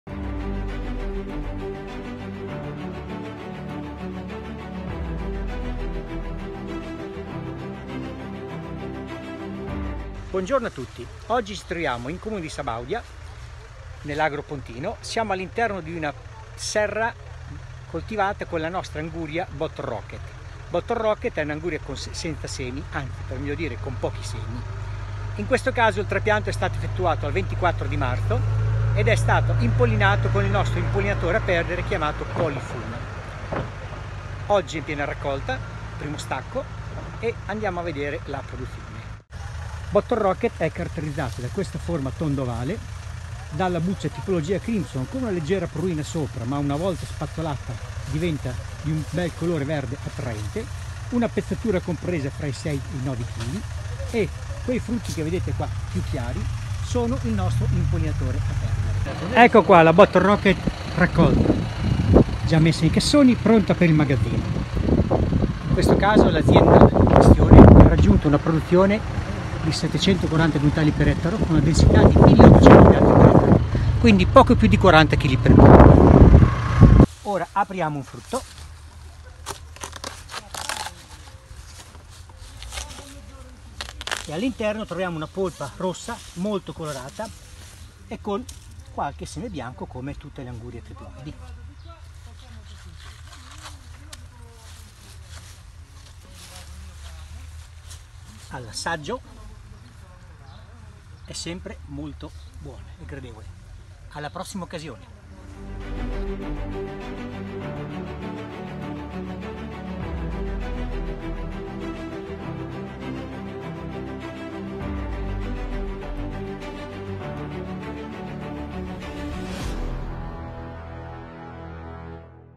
Anguria Bottle Rocket - striatura Crimson e nessun seme /// Seminis by Bayer
Bottle Rocket è l'anguria senza semi più amata della penisola: agricoltori e consumatori ne vanno matti!

Le sue caratteristiche distintive sono la forma tono ovale, la striatura di tipo Crimson e la polpa rossa e dal gusto zuccherino.

Vanes Montanari, Responsabile Tecnico Anguria di Seminis Italia è andati in serra a Sabaudia per non perdersi il primo stacco della raccolta di Bottle Rocket.